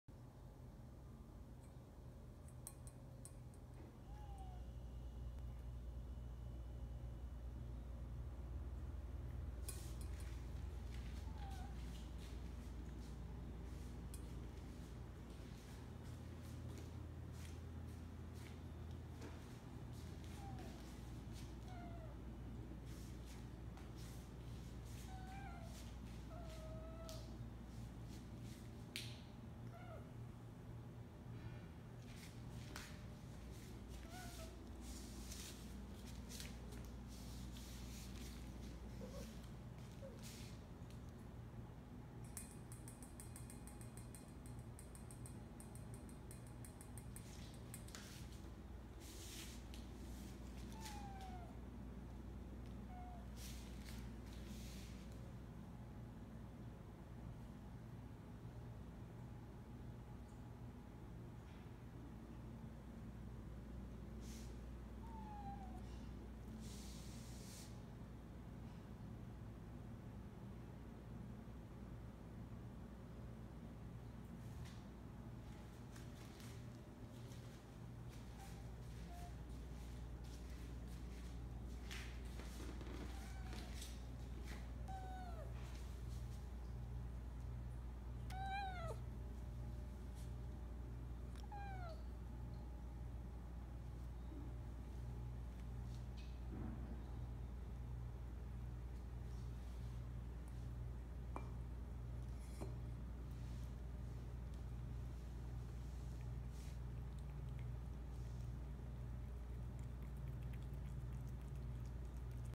Siberian Meows for Food
Teased my cat with her food this morning to catch her meowing on camera :)